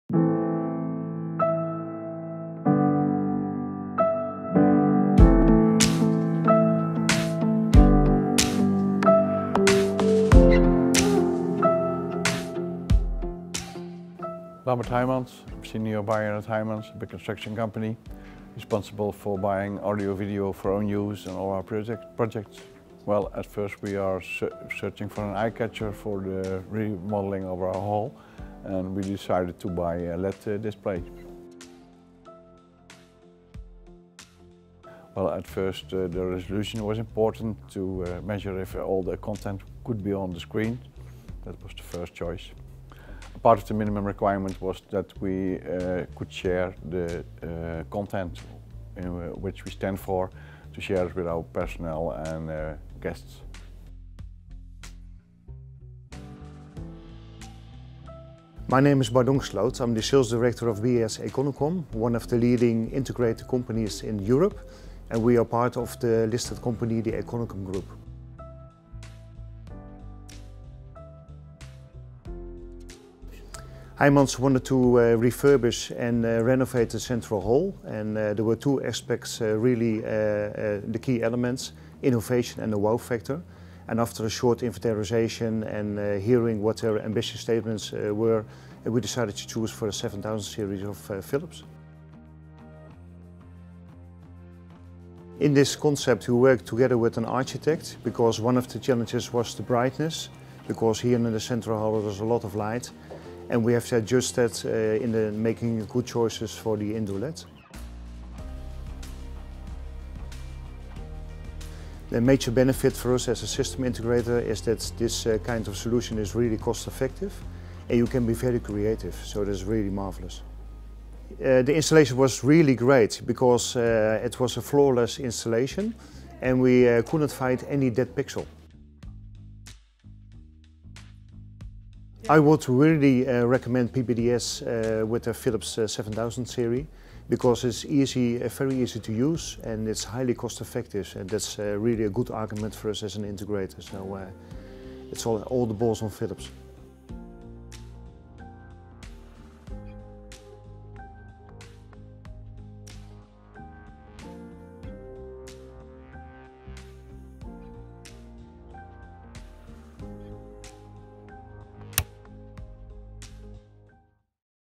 Indoor LED wall Phillips 7000 series at Heijmans
Bouwgigant Heijmans is al jarenlang klant van BIS|Econocom voor de inrichting van verschillende locaties. Onlangs werd de centrale hal op het hoofdkantoor in Rosmalen verbouwd.

Als finishing touch was Heijmans op zoek naar een echte eye-catcher om klanten en medewerkers te informeren en te inspireren.

Met de prachtige LED wall maakt Heijmans een geweldige eerste indruk op bezoekers, waarbij het innovatieve karakter extra onder de aandacht wordt gebracht.

Meer weten over communiceren via grote LED walls? Ga dan naar